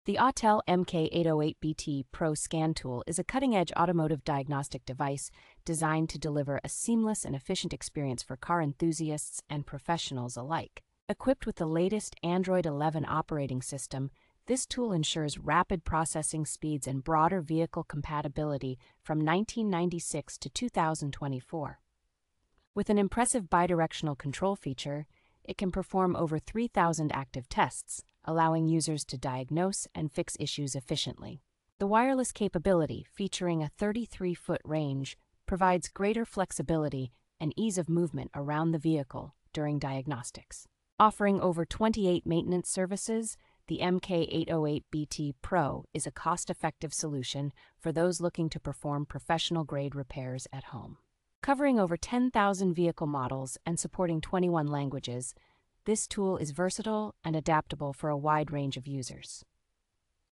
REVIEW (2025): Autel MK808BT PRO Scan Tool. ESSENTIAL details.
The Autel MK808BT PRO Scan Tool is a cutting-edge automotive diagnostic device designed to deliver a seamless and efficient experience for car enthusiasts and professionals alike.

Equipped with the latest Android Eleven operating system, this tool ensures rapid processing speeds and broader vehicle compatibility from nineteen ninety-six to two thousand twenty-four.

With an impressive bidirectional control feature, it can perform over three thousand active tests, allowing users to diagnose and fix issues efficiently.

The wireless capability, featuring a thirty-three foot range, provides greater flexibility and ease of movement around the vehicle during diagnostics.

Offering over twenty-eight maintenance services, the MK808BT PRO is a cost-effective solution for those looking to perform professional-grade repairs at home.

Covering over ten thousand vehicle models and supporting twenty-one languages, this tool is versatile and adaptable for a wide range of users.